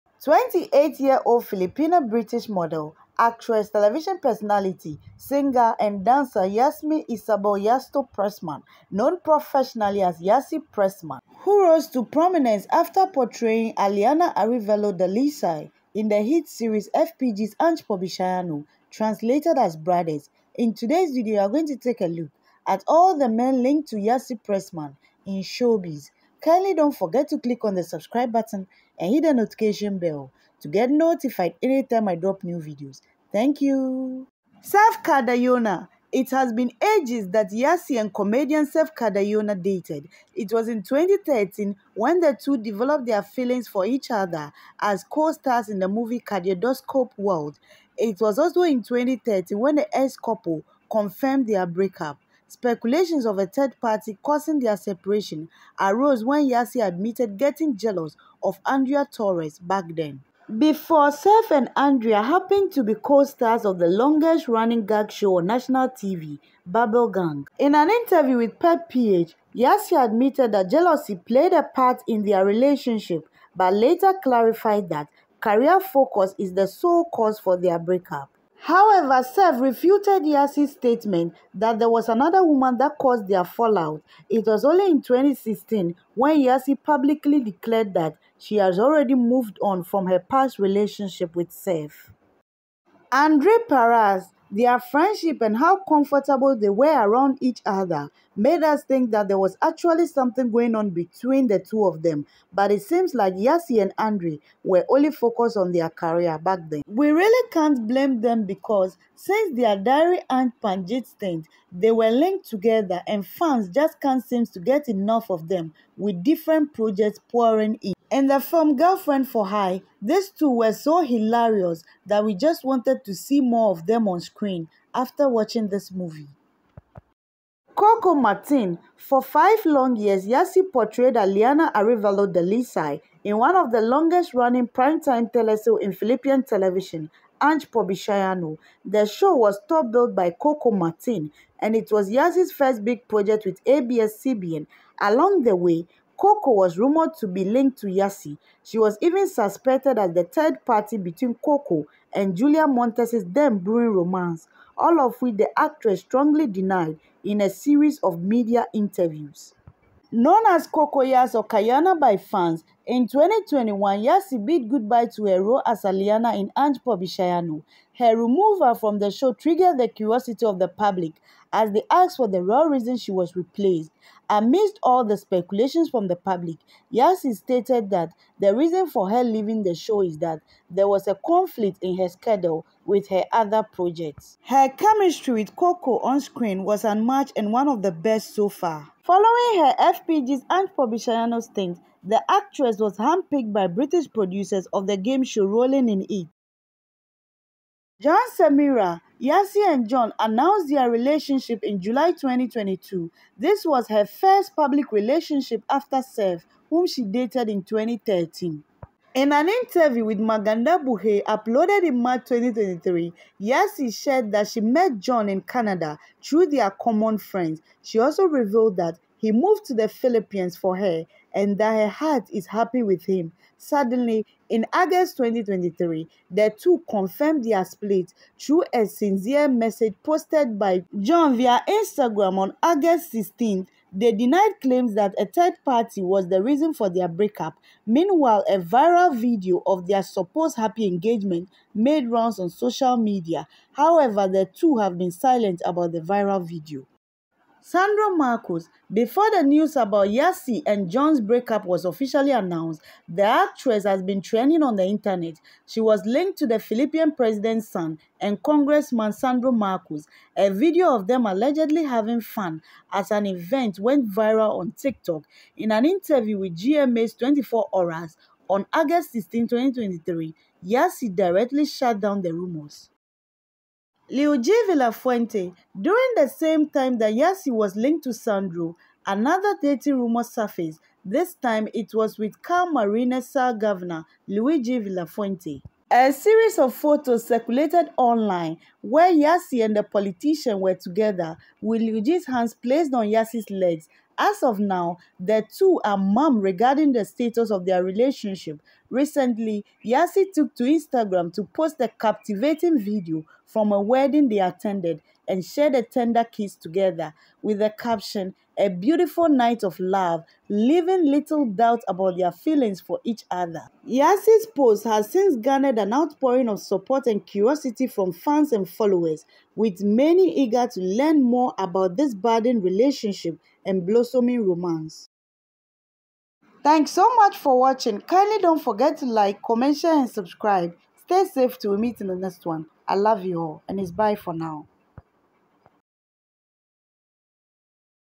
Men Linked to Yassi Pressman || All the Details Revealed
This Video gives the details of all the men linked to Yassi Pressman (All the details Revealed)